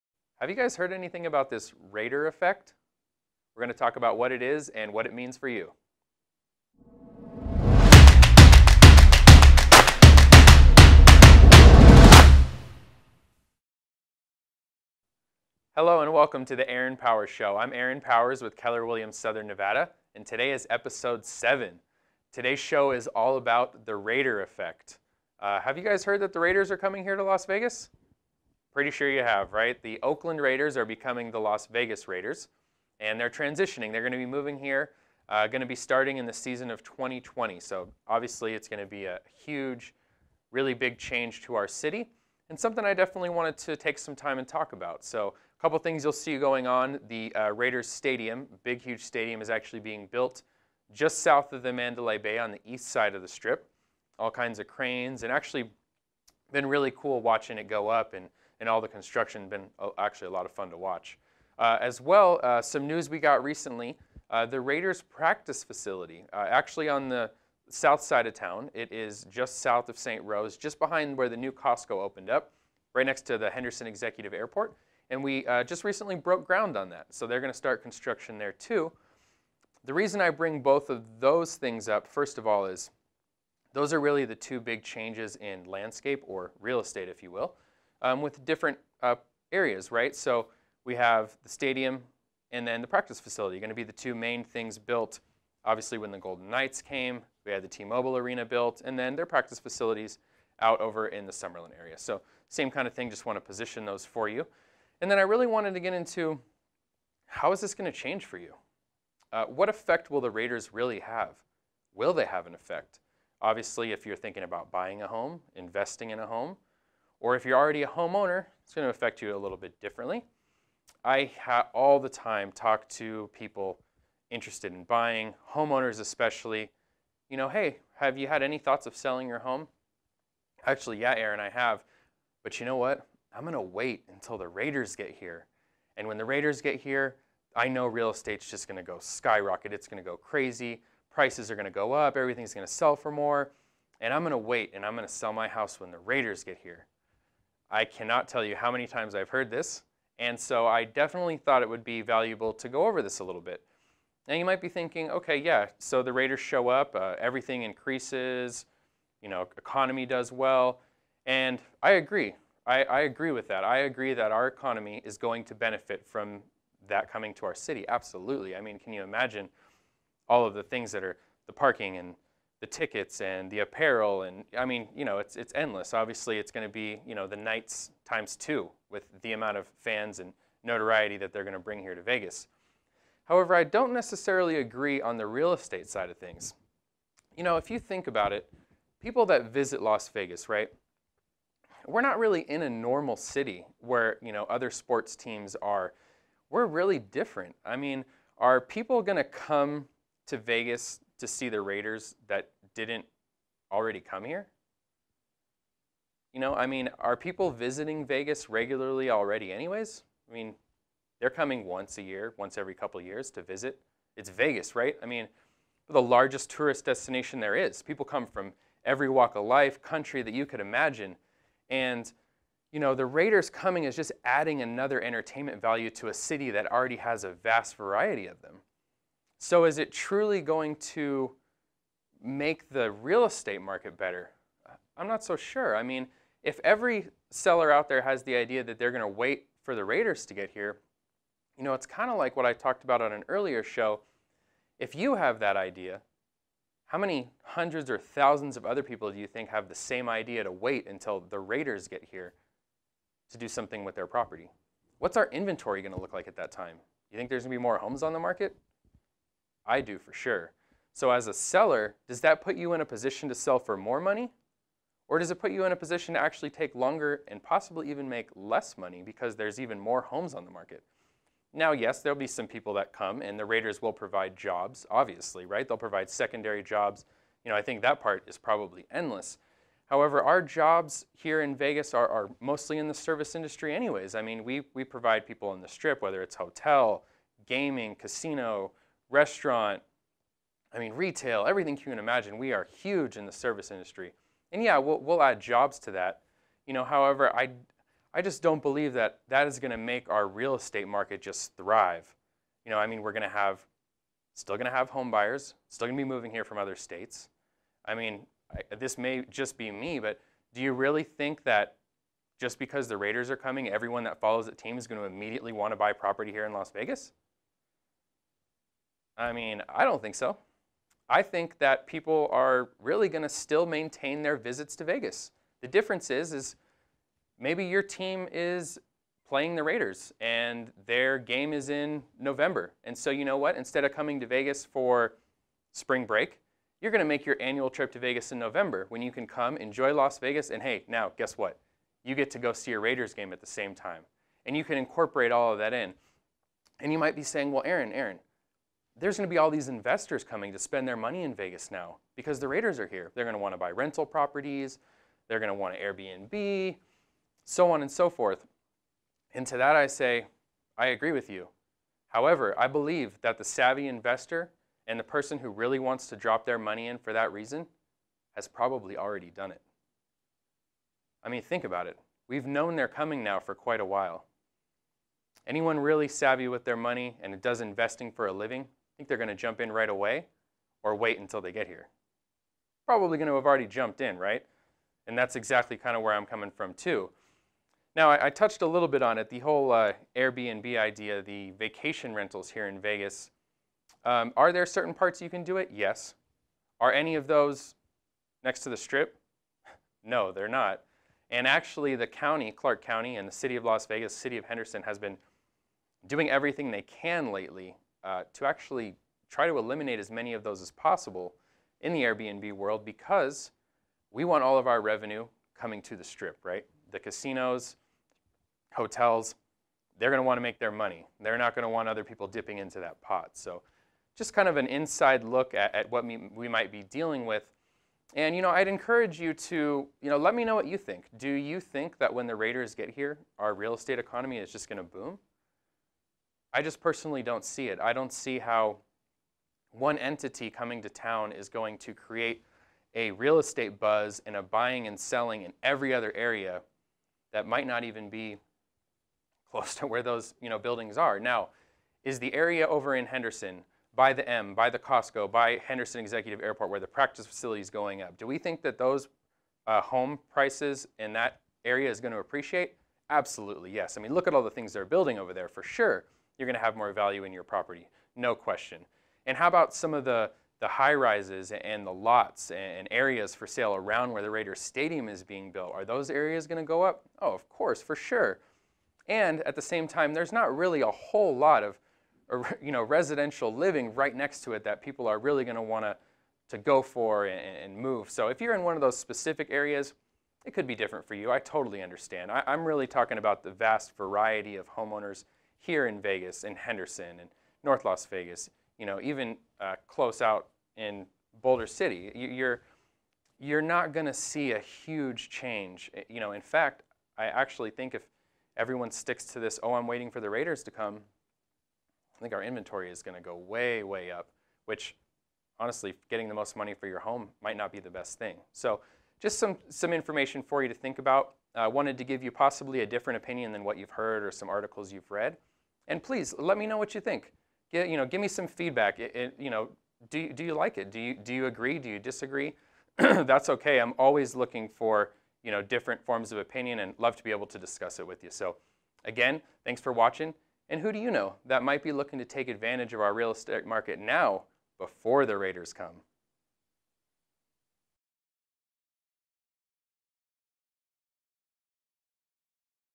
The Raider Effect | Las Vegas Real Estate
Have you heard that the Raiders are coming to Las Vegas?!
Have you heard that Real Estate is going to skyrocket because of it?

We get questions and comments almost daily about the Raiders and their impending relocation to Las Vegas. I am almost certain you have had these same thoughts are even asked some of these same questions. The NFL season of 2020 is fast approaching and it will be here before we know it.

In today's show, Aaron gives some clarity regarding this issue, and what you can actually expect to happen. This is a popular subject that has seen a lot of different discussions and opinions come about. The Raiders are going to have an impact on all parts of our local economy, let's see what this means for our real estate market.

This is a very exciting time to be a Las Vegas resident, as we have been waiting for so many years to see professional sports come to our city. The Golden Knights have been an amazing success and now we have another professional sports team coming as well. Time for the NBA and MLB to jump on board and join us! It's only a matter of time.

AaronPowersShow - Episode 7

Keller Williams Southern Nevada
10424 S Eastern Ave, Ste 200